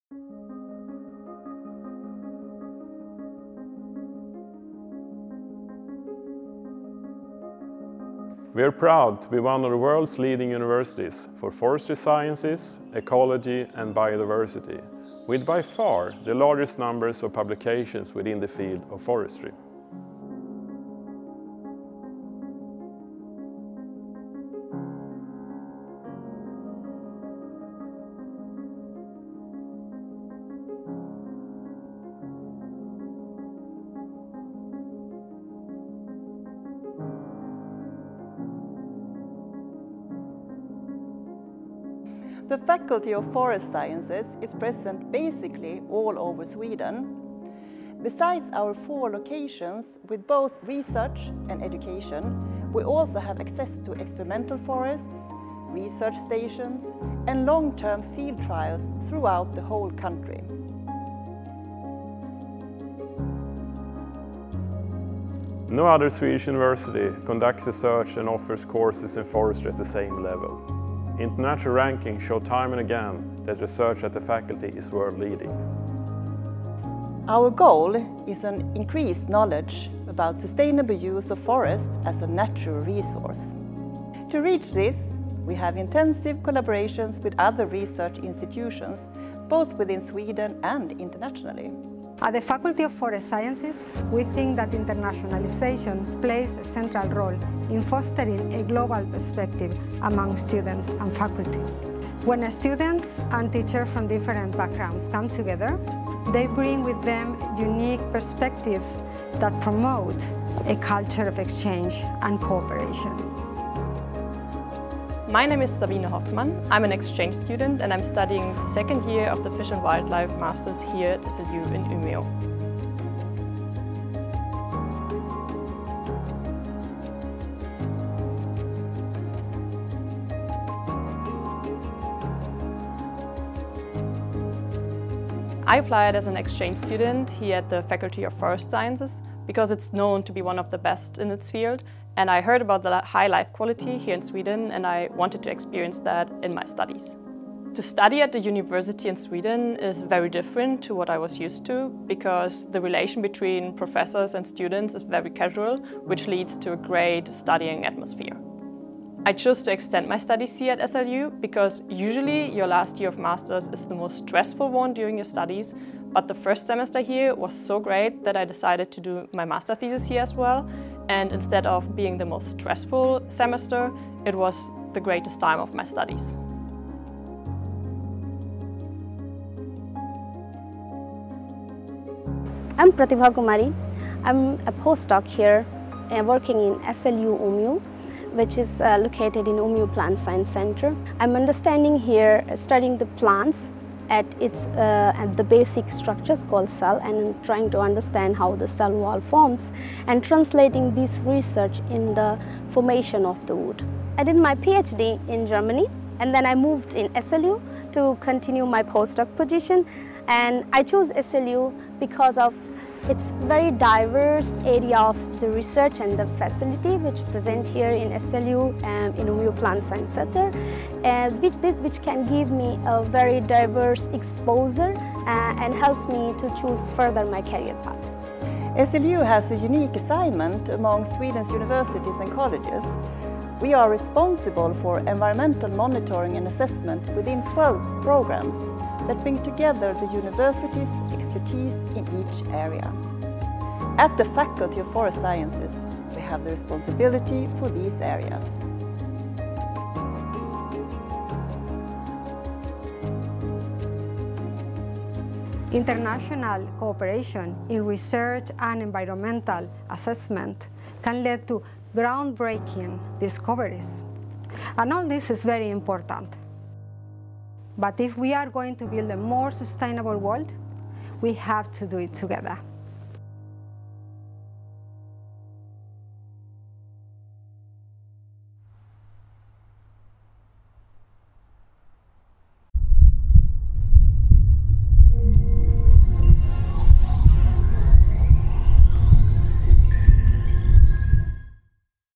The Movie about Faculty of Forest Sciences at SLU
A movie about the Faculty for Forest Sciences at SLU - The Swedish University of Agricultural Sciences: The Faculty of Forest Sciences occupies a unique position. No other Swedish university conducts research and offers educational courses in forestry at the same level. The faculty also holds a strong position internationally, with by far the largest number of publications within the field of forestry.